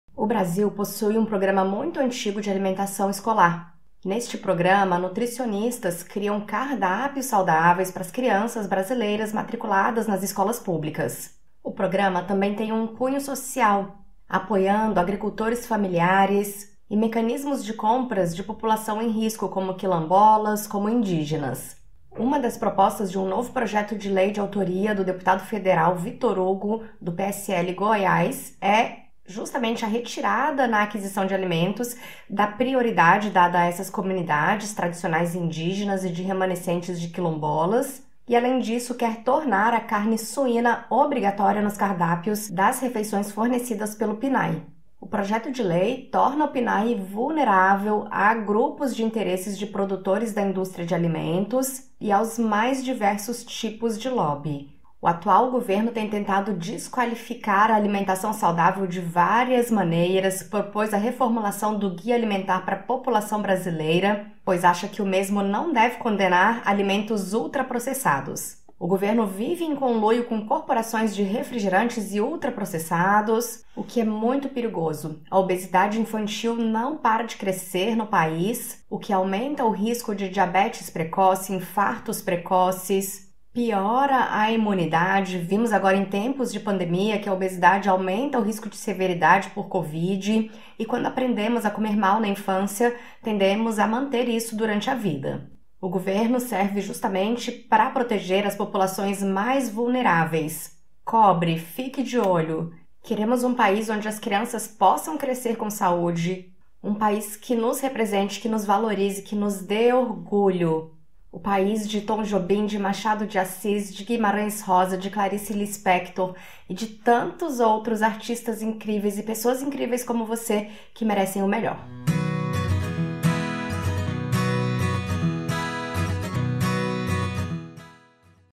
PROGRAMA NACIONAL DE ALIMENTAÇÃO ESCOLAR EM RISCO
O Programa Nacional de Alimentação Escolar (PNAE) tem como objetivo “contribuir para o crescimento e o desenvolvimento biopsicossocial, a aprendizagem, o rendimento escolar e a formação de hábitos alimentares saudáveis dos alunos, por meio de ações de educação alimentar e nutricional e da oferta de refeições que cubram as suas necessidades nutricionais durante o período letivo” (BRASIL, 2009). Dois projetos de lei ameaçam o Programa Nacional de Alimentação Escolar (PNAE), deixando-o vulnerável a todo tipo de lobby da indústria de alimentos. DEFENDA O PNAE!

➤ AVISO

➤ APRENDA MAIS

➤ BIOGRAFIA